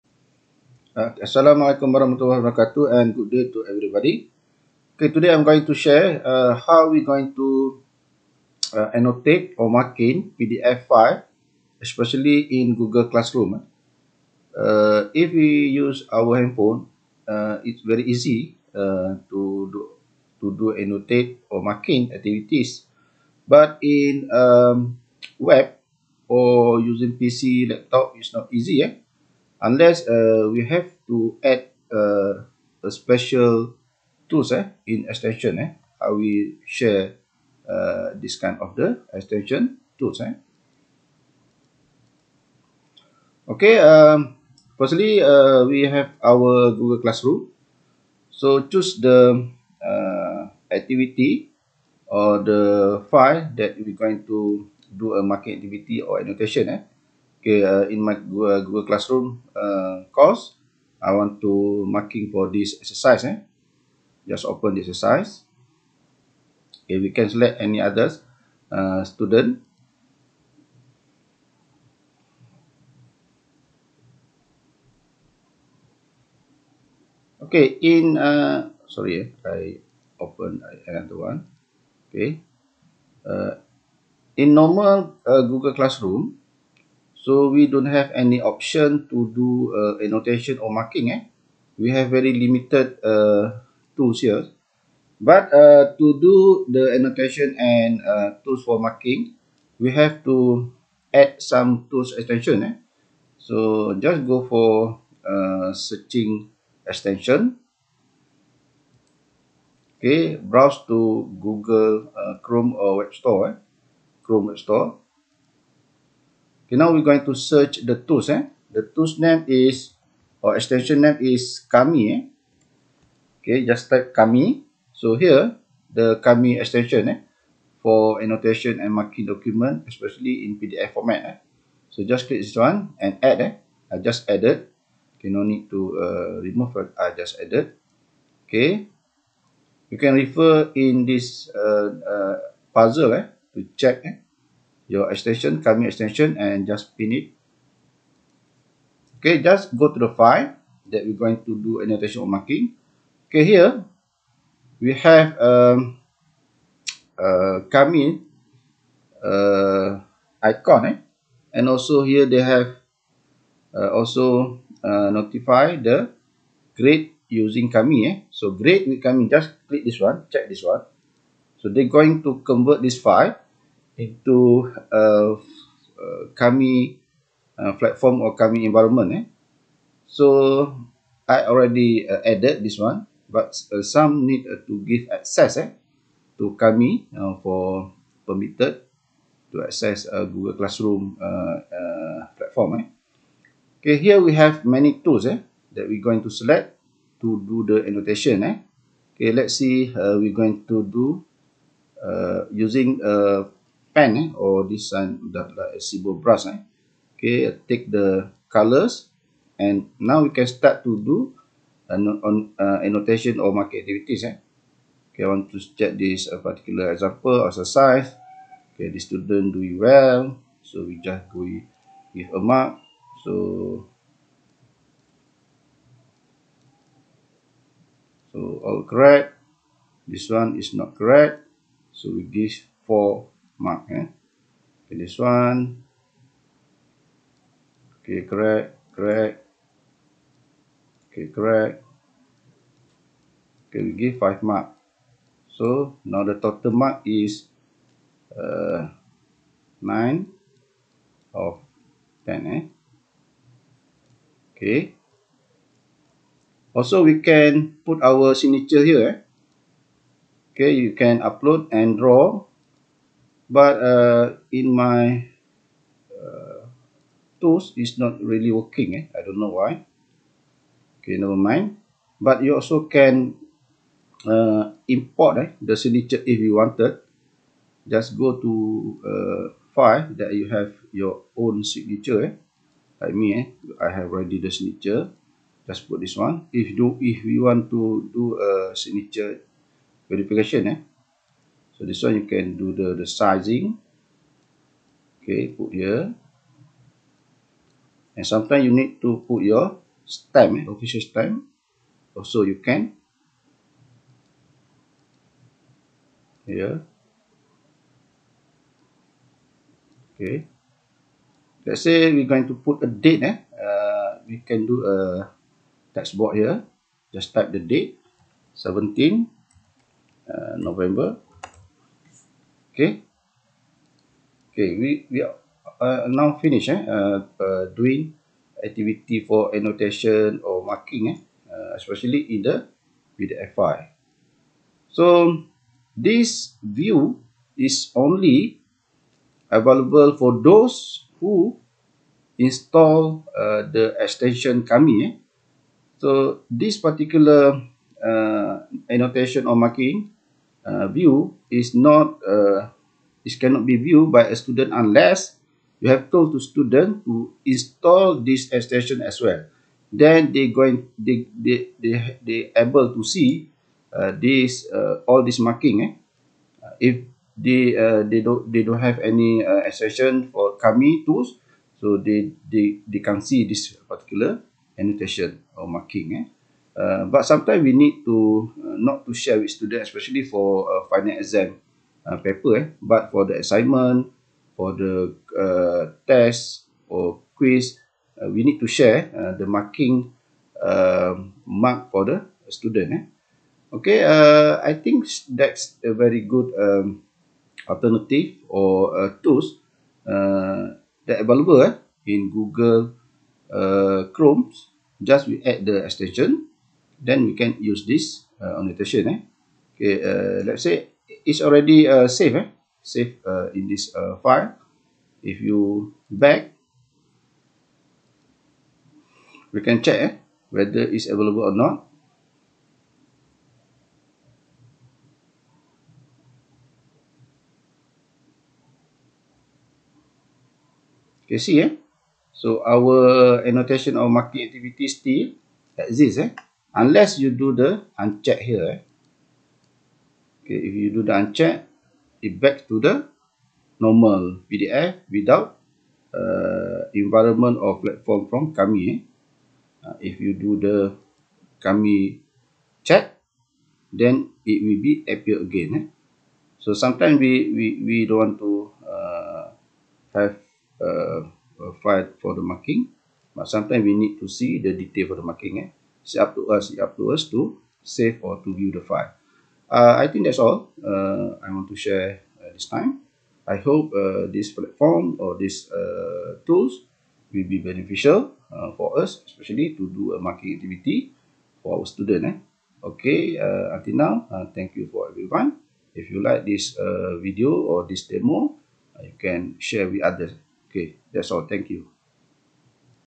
How to Annotate/Marking on PDF in Google Classroom
This video sharing how to do annotation and marking on PDF documents in the Classroom using Kami extension tools. Usually, it is easy to marking or annotate PDF files if we are using a smartphone to operate Google Classroom. However, if we are using a laptop/PC to operate Google Classroom and open a PDF file, there is no special tool marking available, unless we need to install another extension tool called KAMI.

Produced by:
Kamarudin Ambak
Universiti Tun Hussein Onn Malaysia (UTHM)